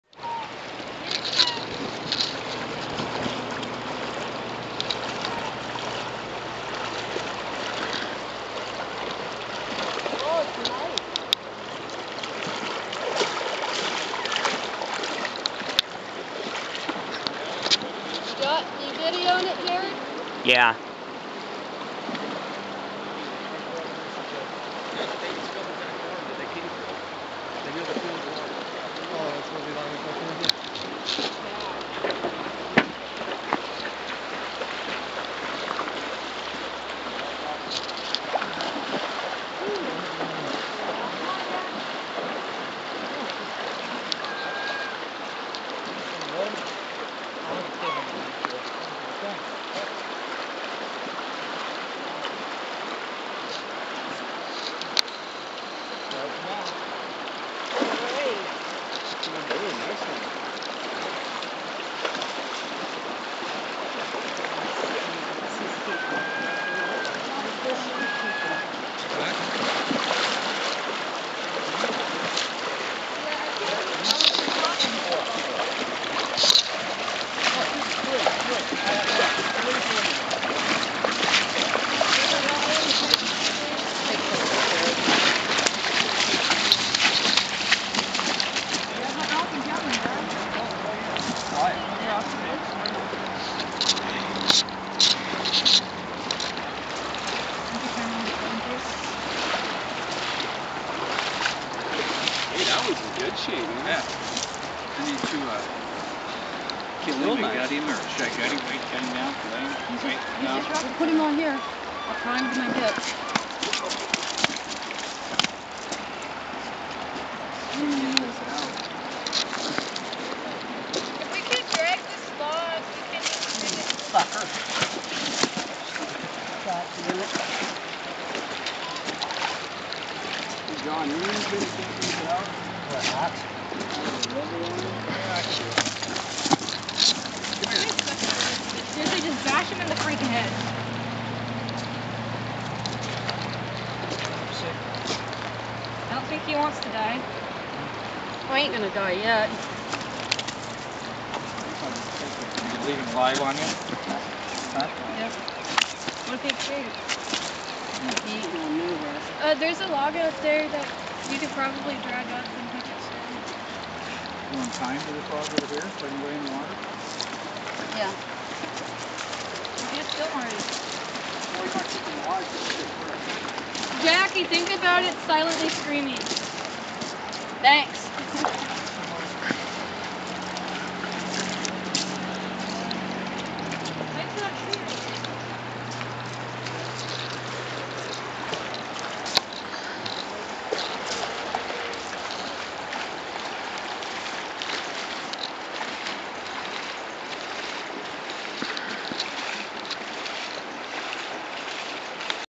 Russian River Reds and Brown bear 005
John, catching his Red, he caught 4 could only keep 3. Awesome day today.